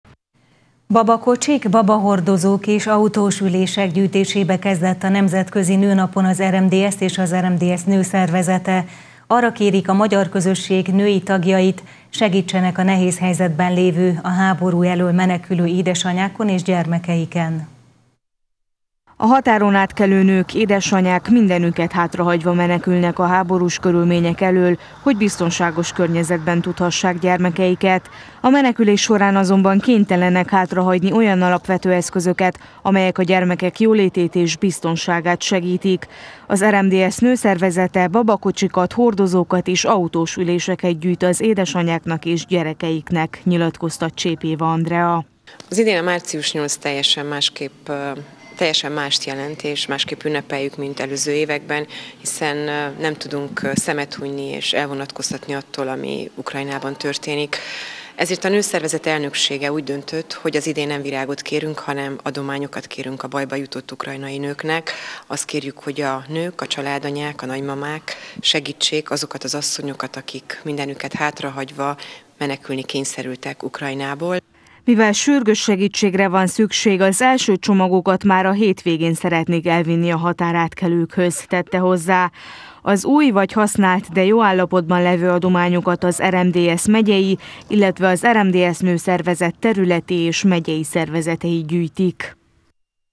Segítség a bajba jutott anyáknak – Erdélyi Magyar Televízió
Babakocsik, babahordozók és autós ülések gyűjtésébe kezd a nemzetközi nőnapon az RMDSZ és az RMDSZ Nőszervezet. Arra kérik a magyar közösség női tagjait, hogy segítsenek a nehéz helyzetben lévő, háború elől menekülő édesanyákon és gyermekeiken.